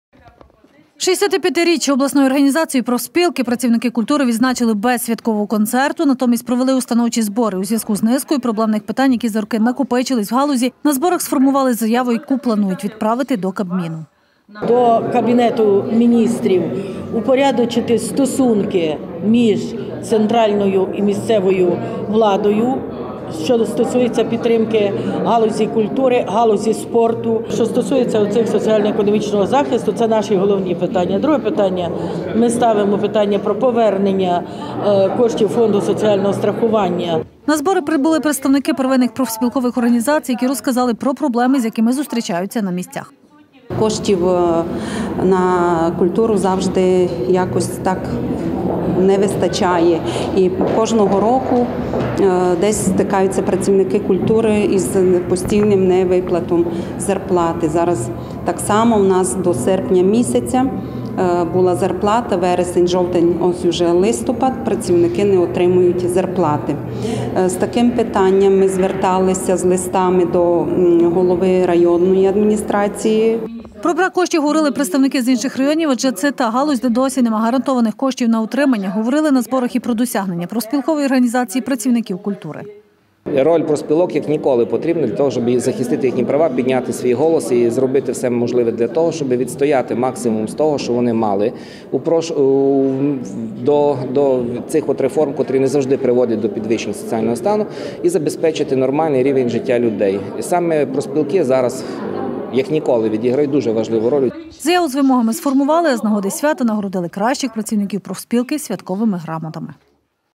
Профспілки святкують
65-річчя обласної організації профспілки працівники культури відзначили без святкового концерту. Натомість провели установчі збори у зв’язку з низкою проблемних питань, які накопичилися в галузі. На зборах сформували заяву, яку планують відправити до Кабміну.